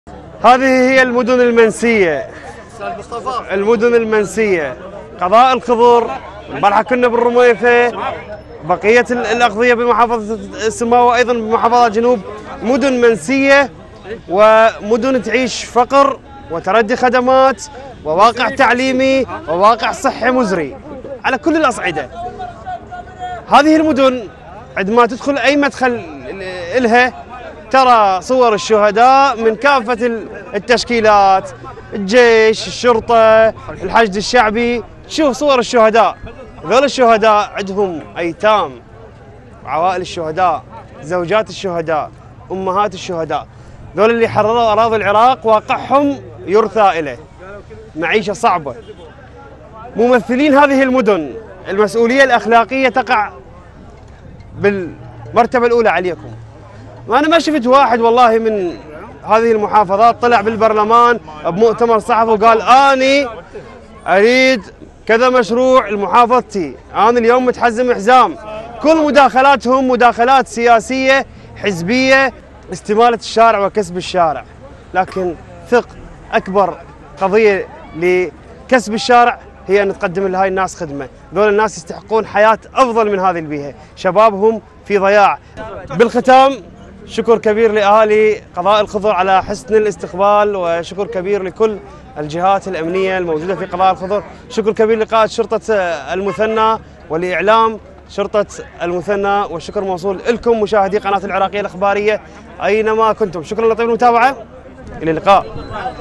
محافظة المثنى ـ حال الخضر كبقية مدن الجنوب التي ضحت بالكثير من الشهداء للدفاع عن كرامة العراق
الوطن والناس مع مصطفى الربيعي
 محافظة المثنى